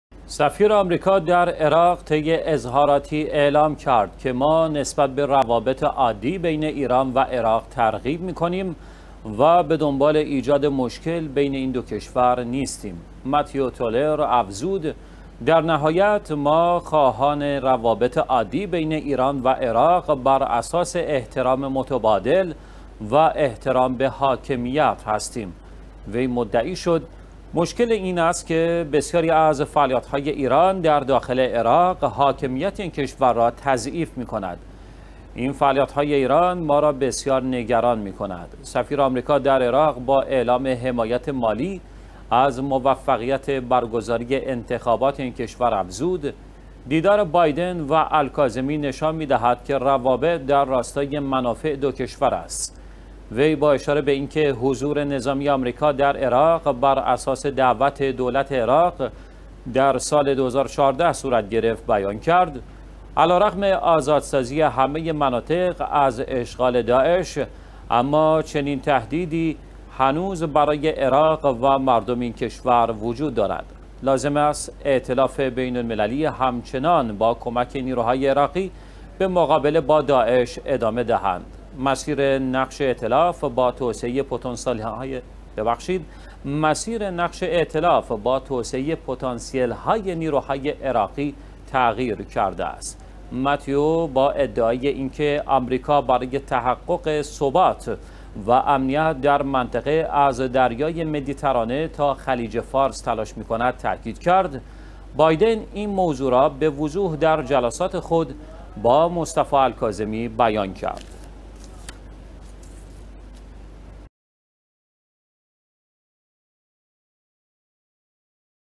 سفیر آمریکا در بغداد: به دنبال ایجاد مشکل بین ایران و عراق نیستیم
سفیر آمریکا در عراق طی اظهاراتی اعلام کرد که "ما نسبت به روابط عادی بین ایران و عراق ترغیب می کنیم و به دنبال ایجاد مشکل بین این دو کشور نیستیم."
"متیو تولر" افزود: در نهایت ما خواهان روابط عادی بین ایران و عراق براساس احترام متبادل و احترام به حاکمیت هستیم.
وی مدعی شد: مشکل این است که بسیاری از فعالیت های ایران در داخل عراق، حاکمیت این کشور را تضعیف می کند. این فعالیت های ایران ما را بسیار نگران می کند.
سفیر آمریکا در عراق با اعلام حمایت مالی از موفقیت برگزاری انتخابات این کشور افزود: دیدار بایدن و الکاظمی نشان می دهد که روابط در راستای منافع دو کشور است.
وی با اشاره به اینکه حضور نظامی آمریکا در عراق براساس دعوت دولت عراق در سال ۲۰۱۴ صورت گرفت، بیان کرد:علی رغم آزادسازی همه مناطق از اشغال داعش اما چنین تهدیدی هنوز برای عراق و مردم این کشور وجود دارد. لازم است ائتلاف بین المللی همچنان با کمک نیروهای عراقی به مقابله با داعش ادامه دهند. مسیر نقش ائتلاف با توسعه پتانسیل های نیروهای عراقی  تغییر کرده است.
متیو با ادعای اینکه آمریکا برای تحقق ثبات و امنیت در منطقه از دریای مدیترانه تا خلیج فارس تلاش می کند، تاکید کرد: بایدن این موضوع را به وضوح در جلسات خود با "مصطفی کاظمی" بیان کرد.